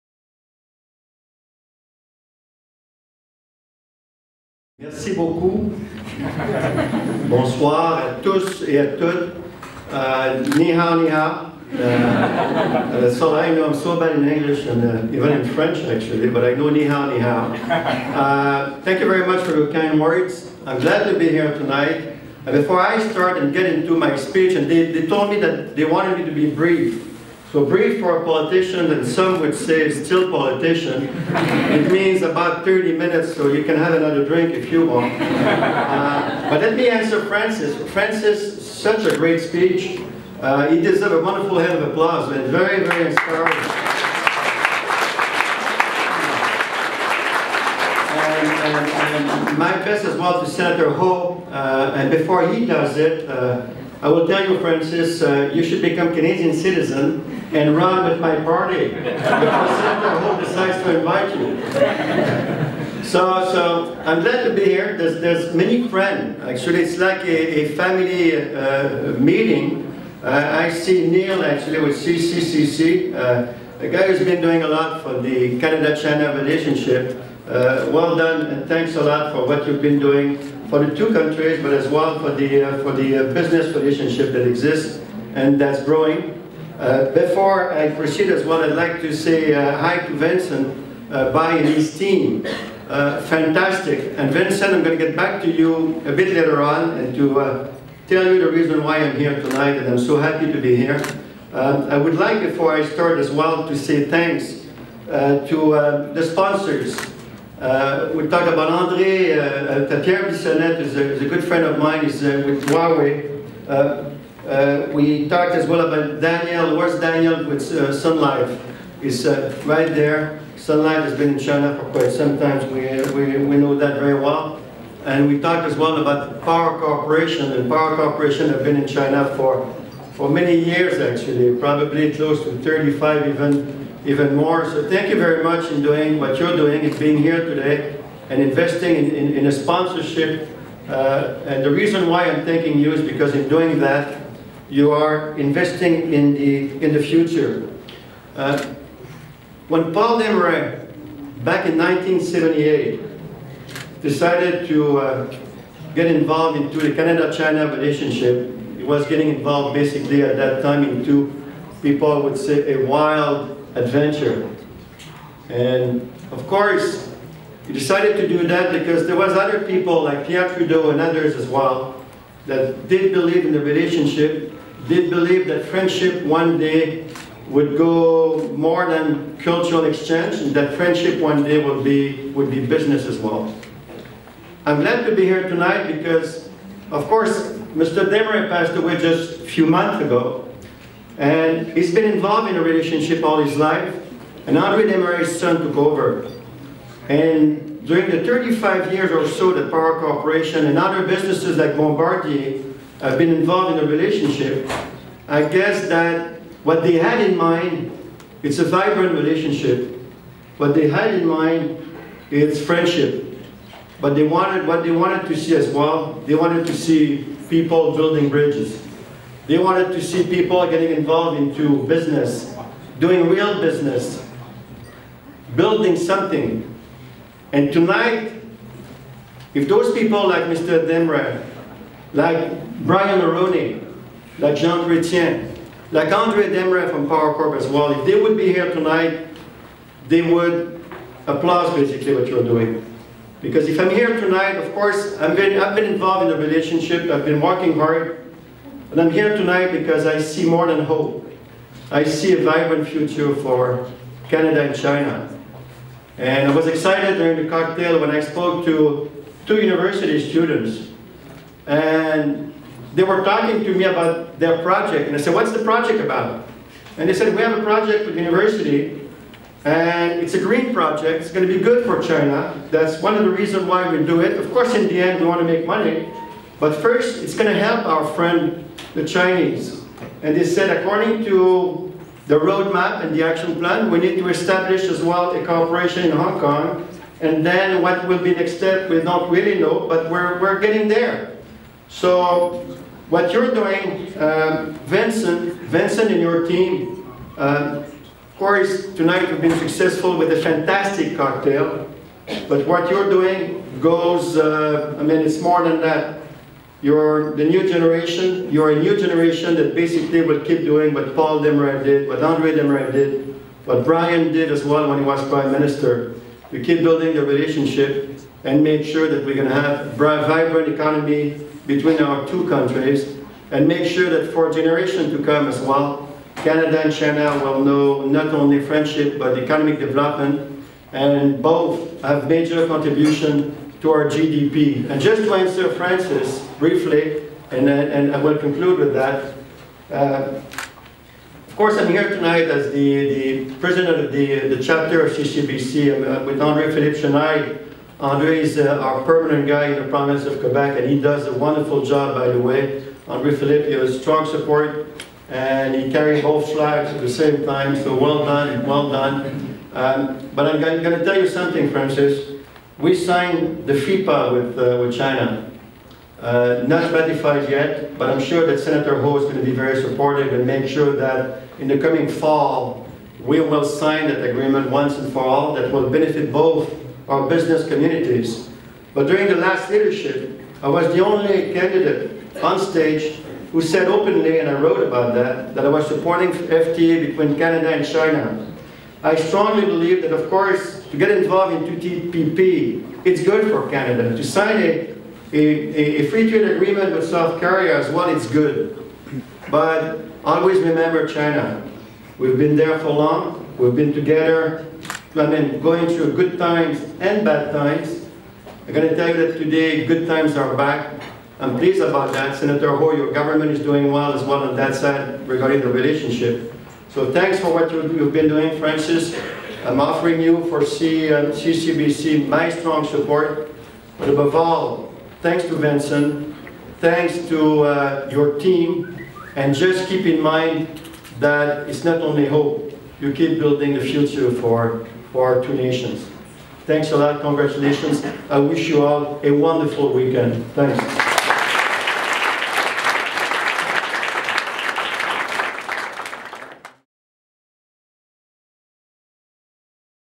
[Canada-China Youth Business Forum] Honourable Martin Cauchon, Vice-Chairman of CCBC
Organizer: China Canada Junior Chamber of Commerce (CCJCC)
Partner: China Canada Chamber of Commerce (CCCC)
Event: CCJCC 2nd Youth Business Forum 2014 - Canada-China 2020: The Next Big Thing
Date: March 21-22, 2014
Location: Intercontinental Hotel, Montreal, Canada

Honourable Martin Cauchon, vice-chairman of Canada China Business Council (CCBC) and representing law firm DS Welch Bussieres, gives a speech at the cocktail night on March 21st 2014.

Martin was first elected to political office in October 1993 in the federal riding of Outremont, Québec. Throughout his distinguished political career, he has held many pivotal cabinet positions, including Secretary of State (Economic Development Agency of Canada for the Regions of Québec) from January 1996 to January 2002, Minister of National Revenue from August 1999 to January 2002, Minister of Justice and Attorney General of Canada from January 2002 to December 2003, as well as Minister with political responsibility for Québec during that same period. Prior to his political career, he practiced civil and commercial litigation from 1985 to 1993. In 1991, Martin completed a Master of Law in International Business Legal Studies at the University of Exeter, in England.